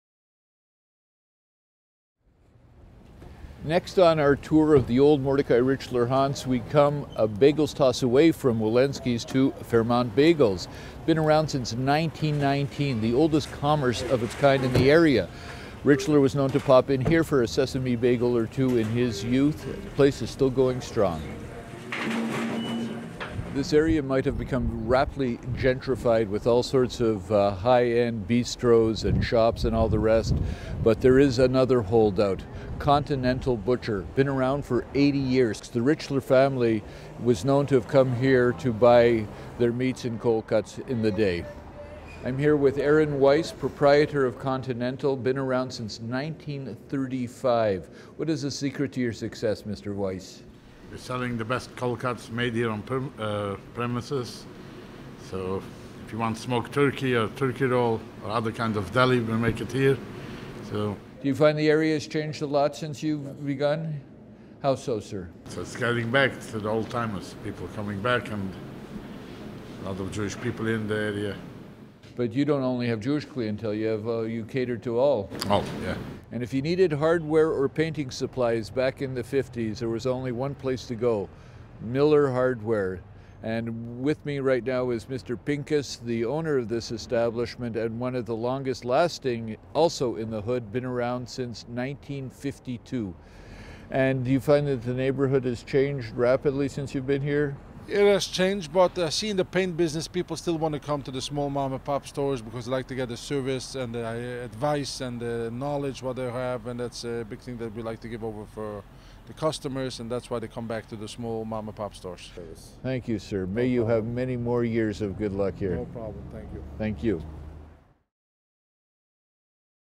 Additional Mordecai Richler spots
Montreal Gazette columnist Bill Brownstein gives a tour of the locations in the neighbourhood of Mile-End in Montreal with connections to the famed montreal writer Mordecai Richler. Video shot on Thursday, May 21, 2015. (Dario Ayala / Montreal Gazette)